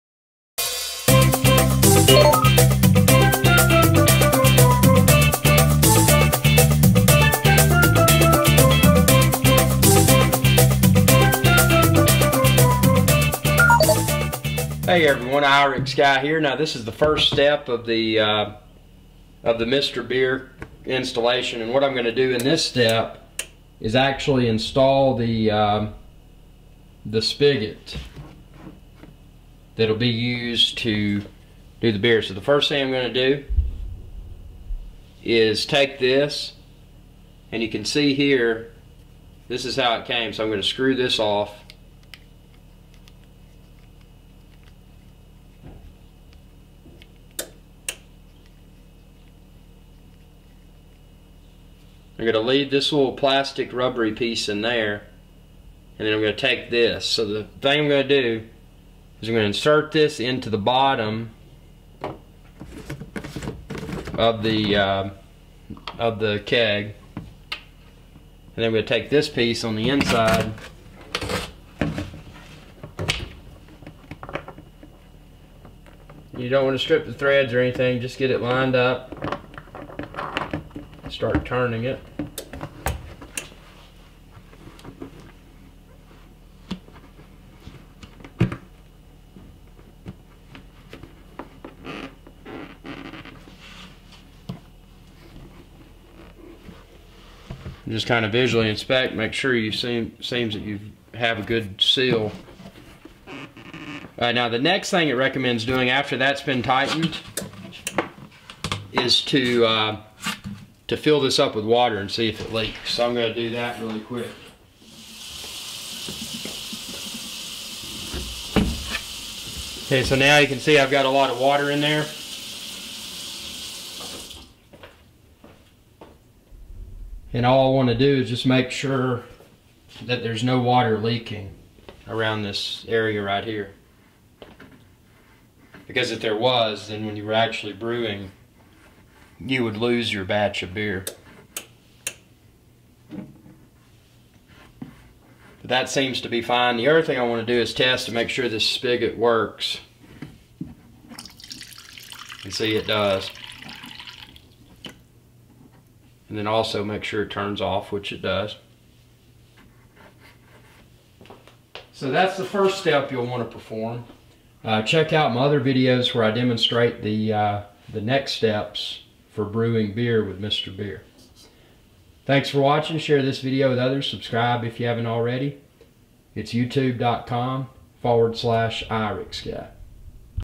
Mr. Beer Assembly Demonstration
Mr. Beer is an excellent kit for anyone interested in brewing beer at home.  The beer brewing process is quite simple and there are an abundance of Mr. Beer varieties available.  The Mr. Beer kit is reusable and so are the beer bottles!  That's what makes Mr. Beer so awesome!  Just be sure to follow the Mr. Beer cleaning instructions to eliminate bacteria that can distort the quality of the beer brewed.